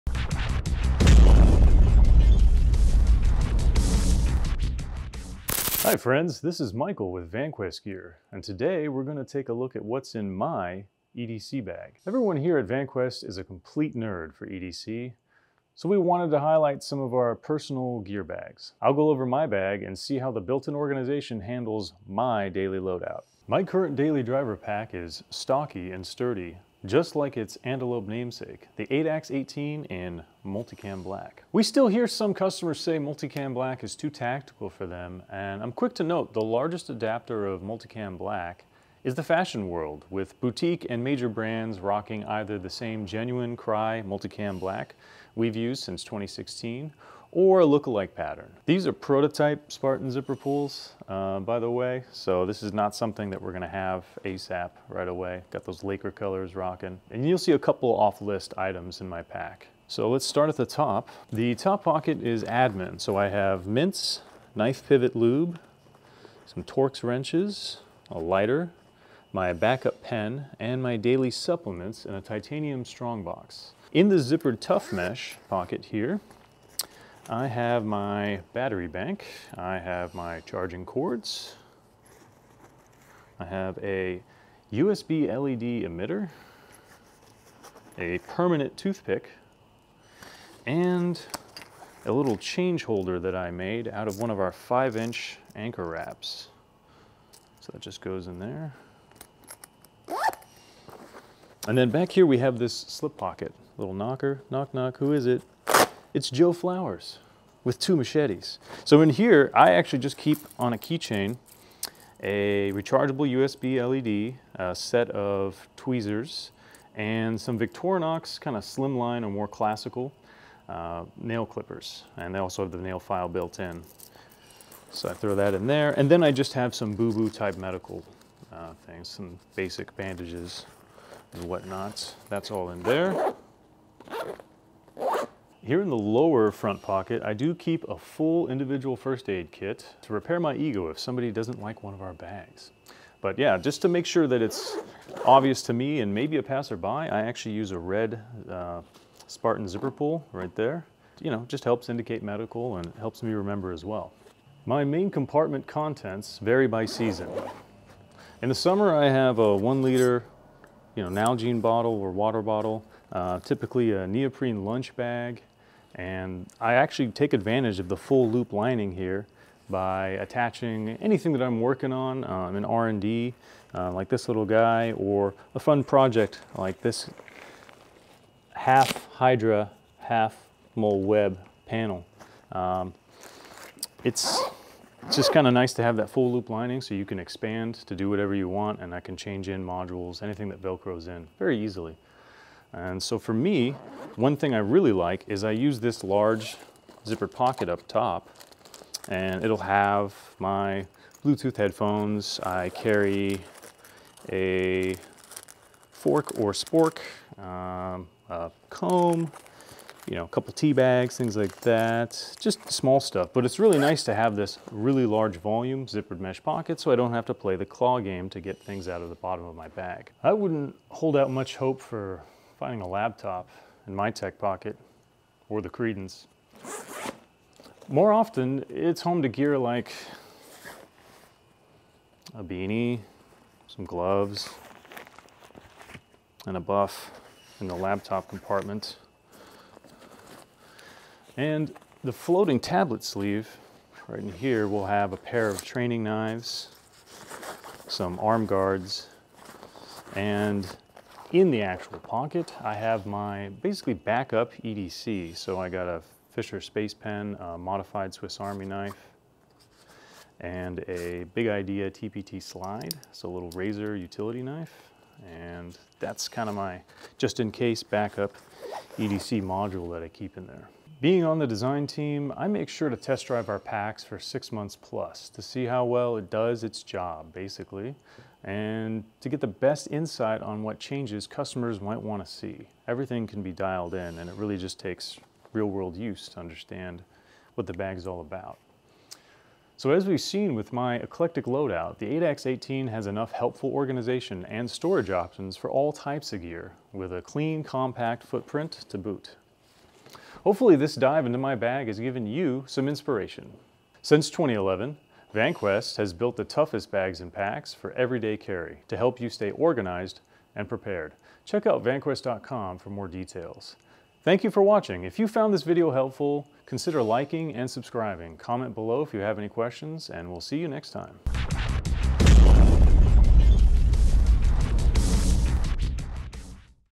What's in my bag? EDC Bag Dump With Mike!
Let's talk EDC with our Account Manager and R&D team member Mike! We dive into his Addax-18 and see what he carries daily!

Checkout for Pre-Order Specials and Product Updates!

Since 2011, Vanquest has built the toughest bags and packs for EVERYDAY CARRY, to help you stay organized and prepared